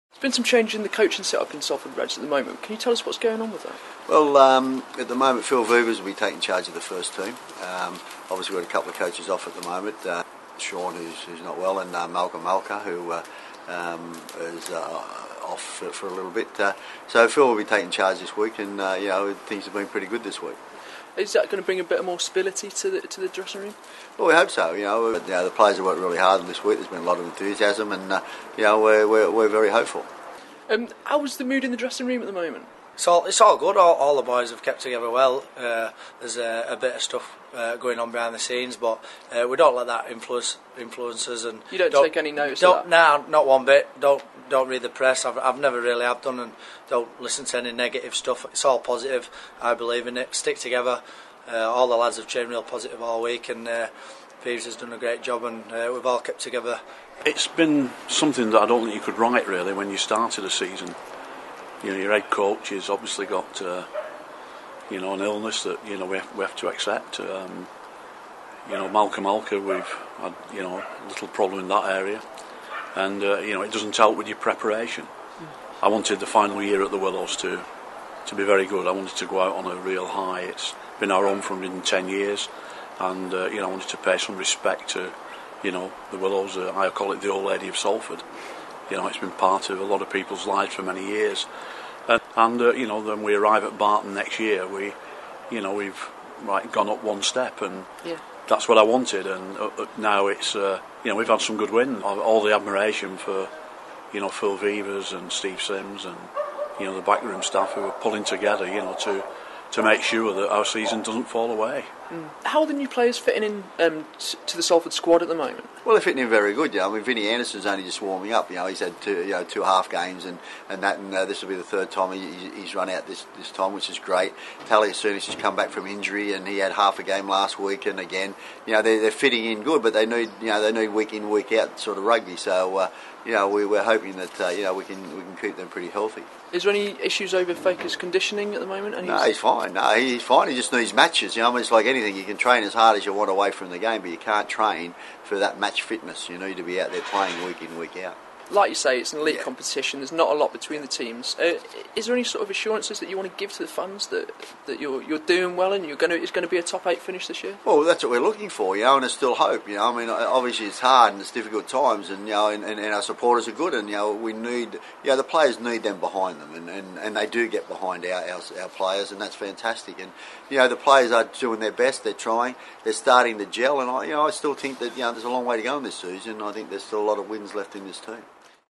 New coach brings life to stilted Salford
With all the fuss surrounding Salford Reds' backroom it was a surprise to see them bounce back against the Bradford Bulls, winning by a clear margin of 56 points to 16.

Coming off the back of two straight losses, Salford needed to show a lot of character to pull off a win against the Bulls.

Phil Vievers has clearly been a stabilising influence on the Reds dressing room - which was left reeling by the loss of head coach Shaun McRae to illness, then stalwart assistant coach Malk Alker's dispute with the club.

With a glut of new signings in the pre-season, Salford were expected to come out strong in the Super League. But there have been questions over the fitness of some players, including Feka Palea'aesina. Acting head coach Steve Simms explains how these new players have been fitting in.

Hooker Wayne Godwin, one of the club's 2011 signings from this week's opponents Bradford, tells us about the mood behind the scenes at the moment.

Finally, Chairman John Wilkinson explains his vision for a great final season at The Willows "so that we go to [the new stadium at] Barton having gone up a step".